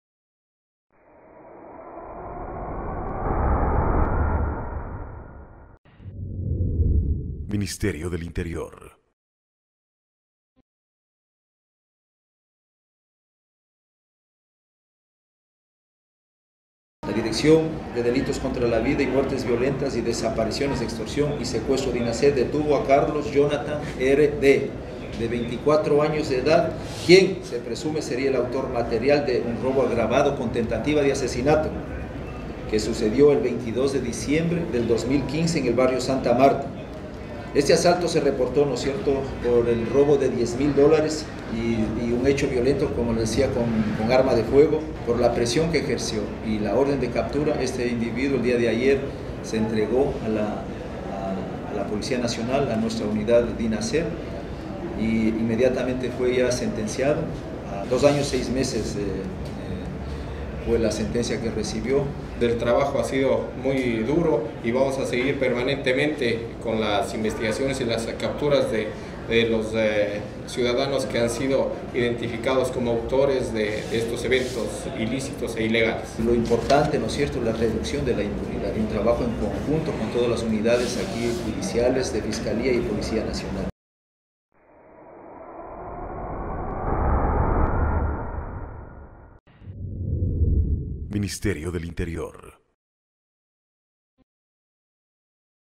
Presunto autor de robo agravado y tentativa de asesinato fue detenido, en Manta
Manta. La Policía Nacional, por medio de la Dirección Nacional de Delitos Contra la Vida, Muertes Violentas, Desapariciones, Extorsión y Secuestro (Dinased) detuvo a Carlos Jonathan R.D., de 24 años de edad, quien se presume sería el autor material de un robo agravado con tentativa de asesinato.